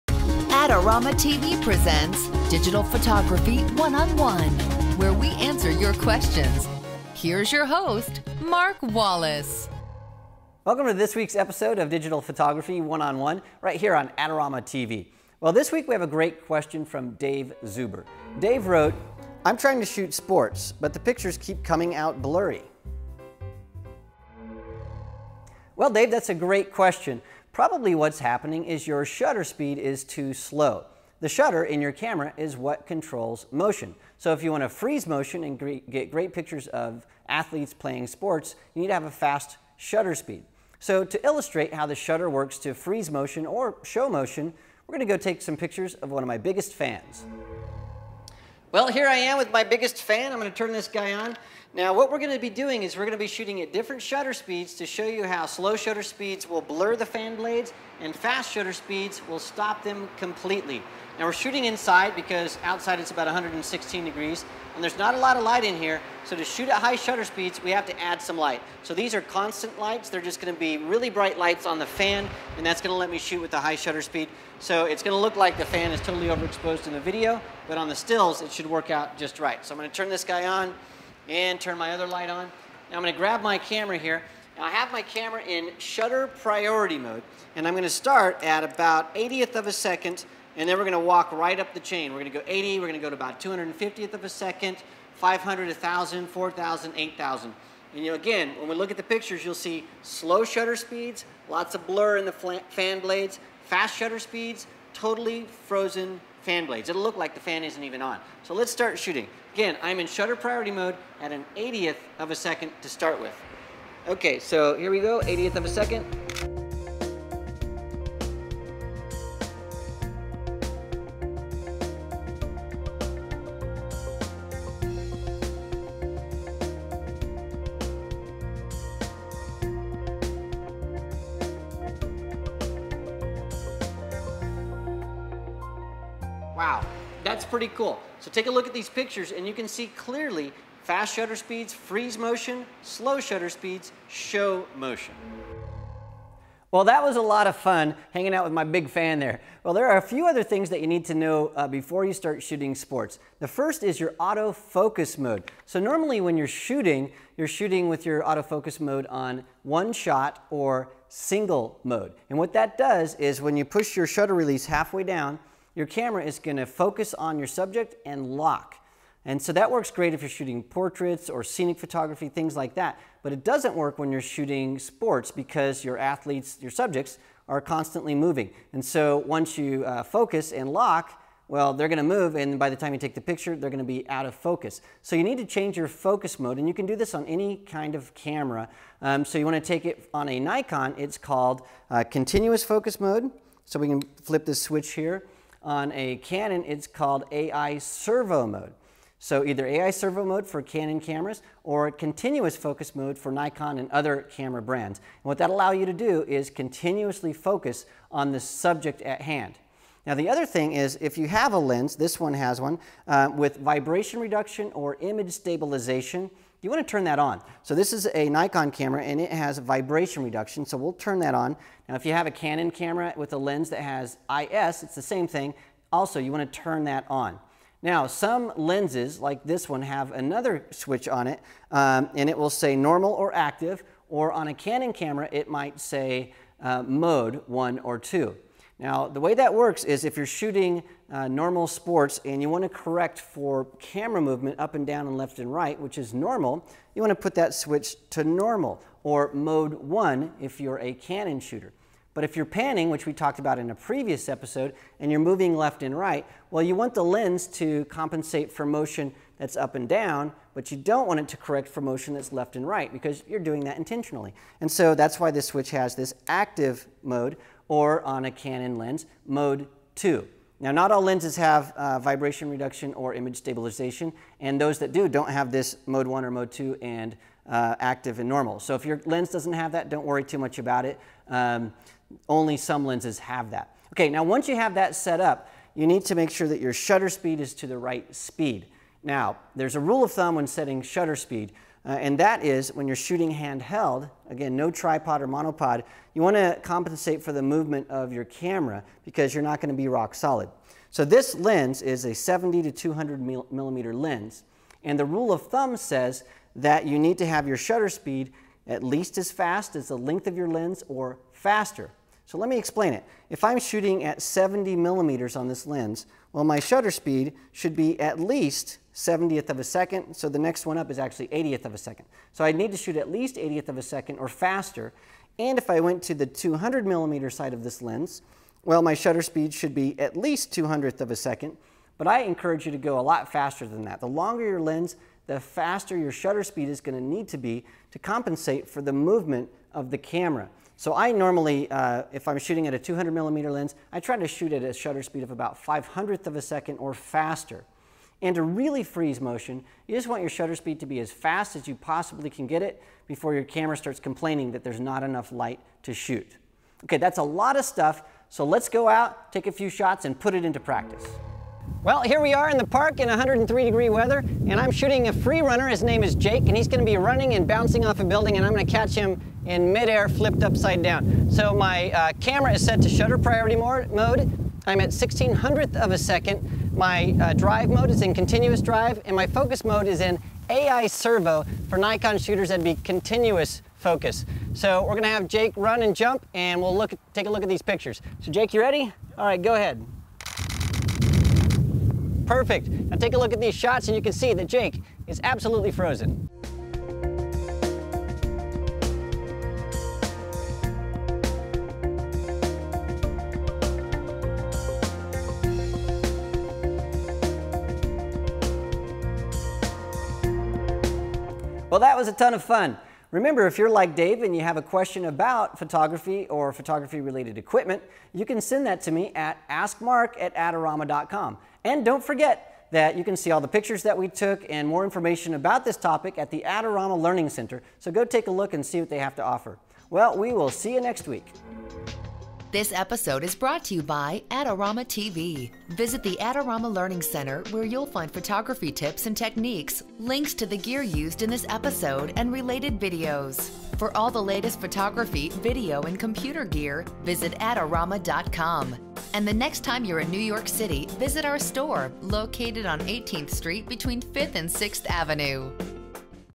Digital Photography 1 on 1: Episode 18: Freezing Motion with your shutter
In this episode Mark talks about using a fast shutter speed to freeze motion. This is a great technique that can be used to capture sports or even kids on the move!